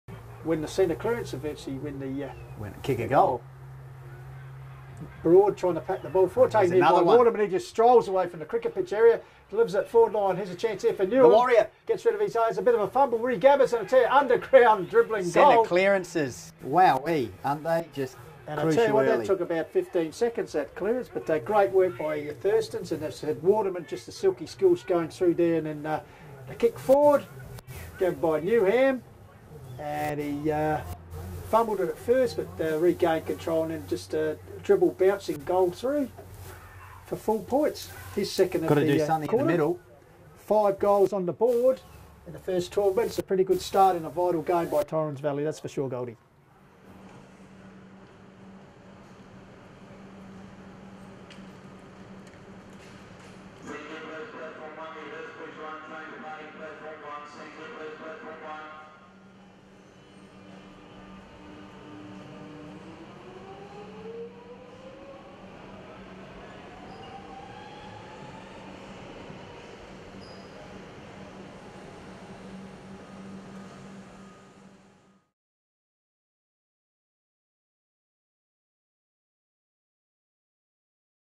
Torrens Valley v Onkas - FootyFlix Fastest Goal of the Year (( contender #2 ))
Josh Newnham beats his own record, set minutes earlier - 13 seconds from leaving the umpire's hand in the centre to crossing the goal line.

* Note that there's a scoreboard error in this video - reverse the scoreline in your head, if you like.